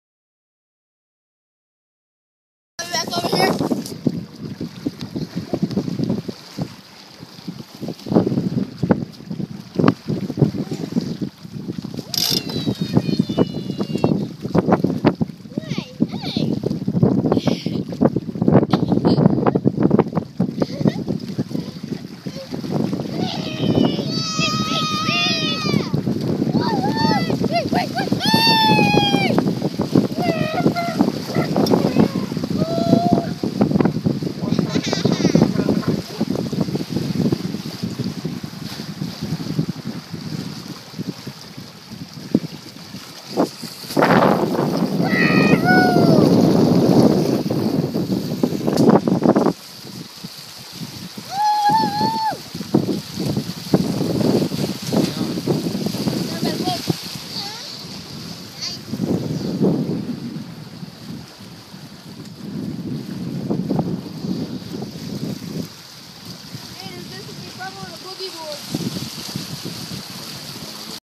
Keelhauling The Crew
Keelhauling:  A form of punishment, used sometimes to improve the morale of the crew.
(They are actually enjoying it!)
Dragging the kids behind the Precision 23 Sailboat.  West Texas